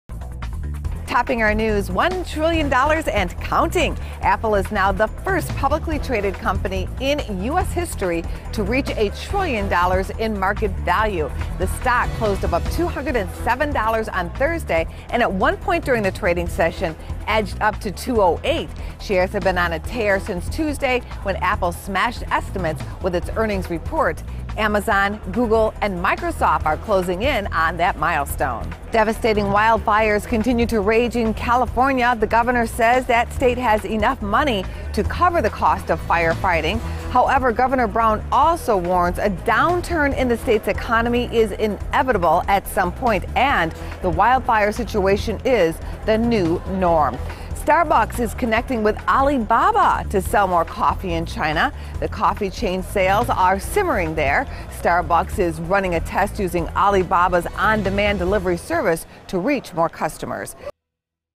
08/03/2018 | Business First Brief
Apple hits a milestone and makes history. Angela Miles reports.